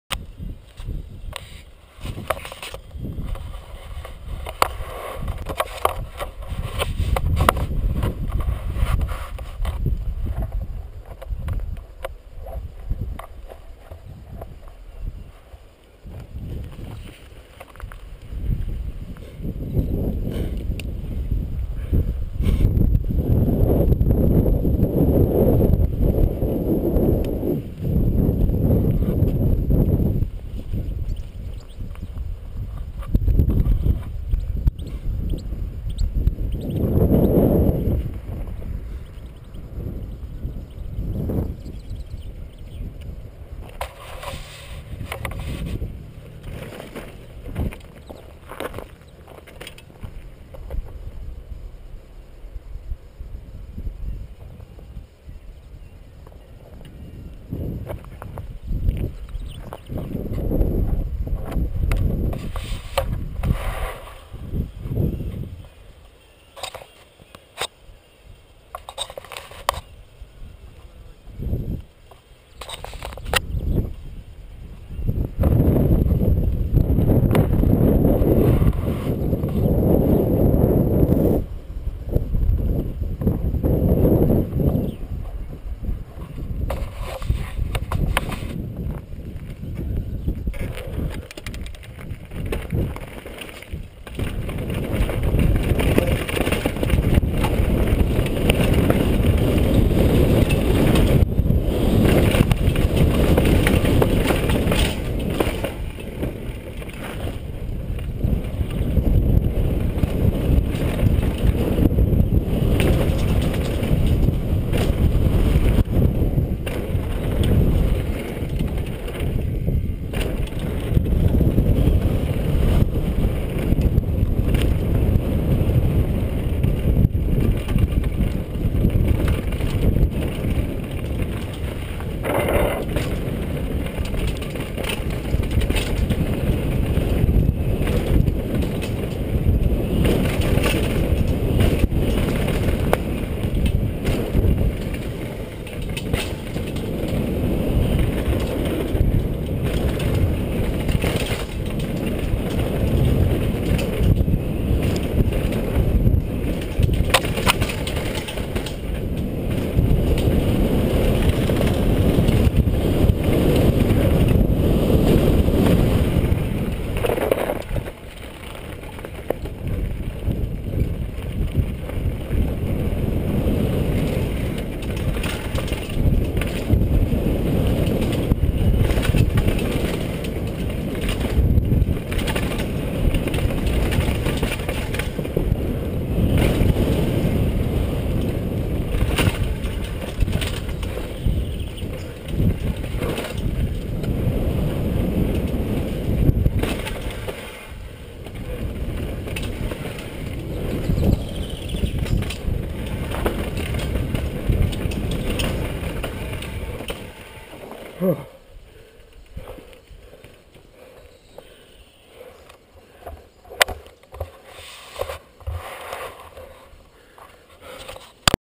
Victoria Park - Go Pro HD
skip to about 1min in for riding, Victoria park dh run with new go pro HD.